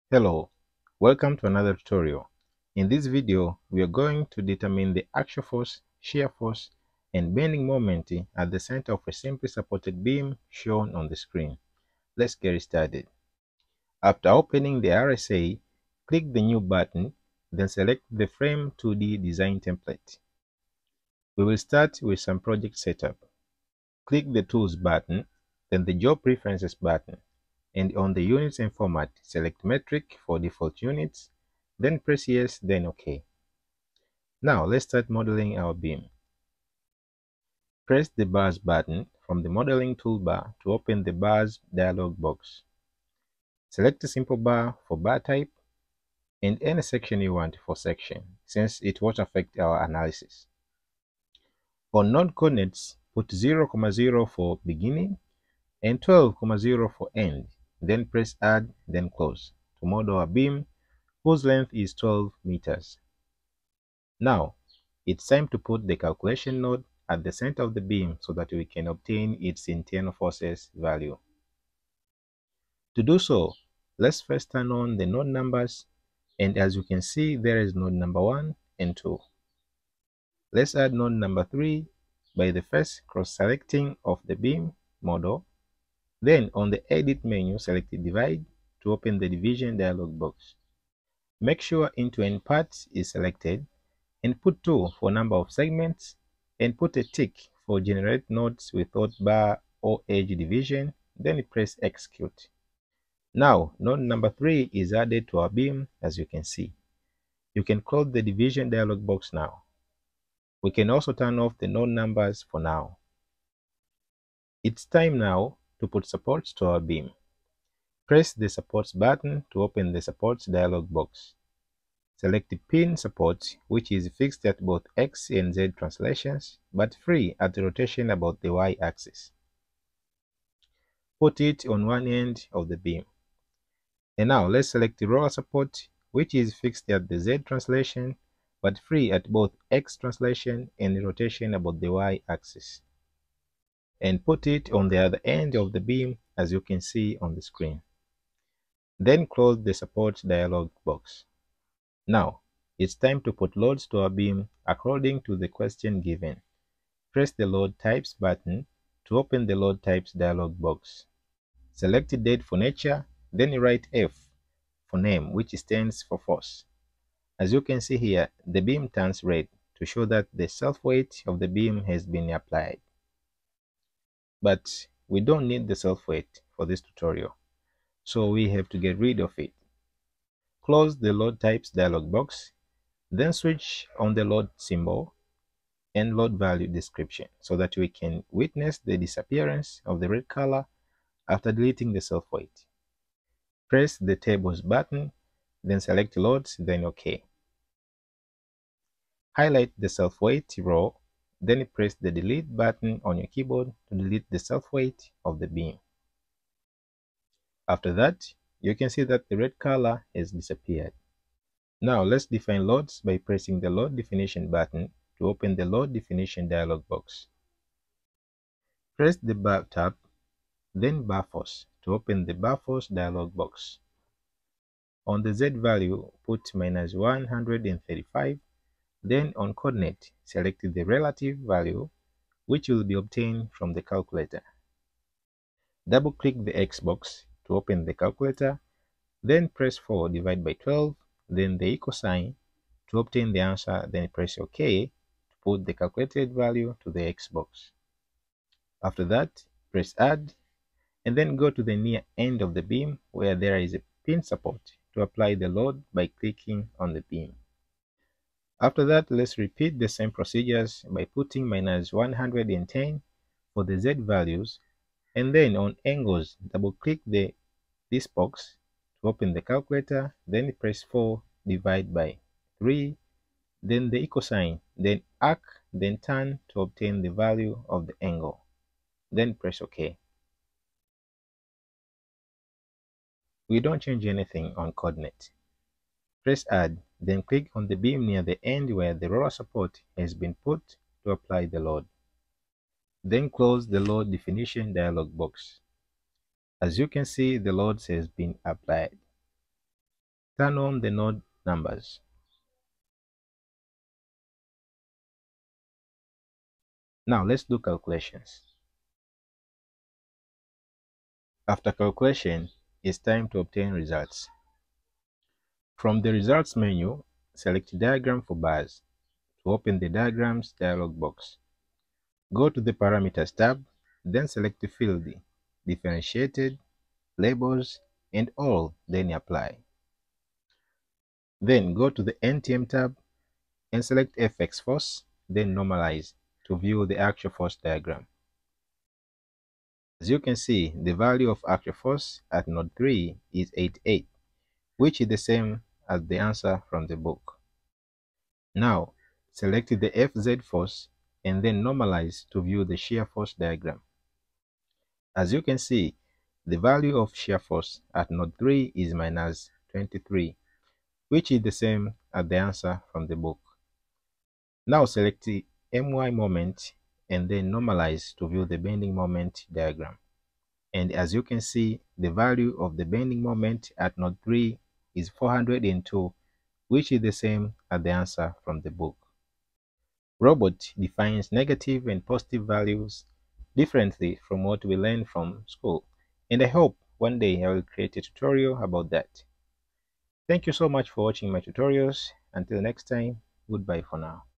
Internal Forces Calculation by Robot Structural Analysis
In this tutorial, you will learn how to calculate the Axial force, Shear force, and Bending Moment of a middle point of a simply supported beam.

Chapters:
00:00 Introduction
00:20 Project Setup
00:33 Modeling Beam
01:03 Setting Calculation Node
01:53 Boundary Conditions
02:33 Load Definition
05:06 Calculations and Results
06:19 Conclusions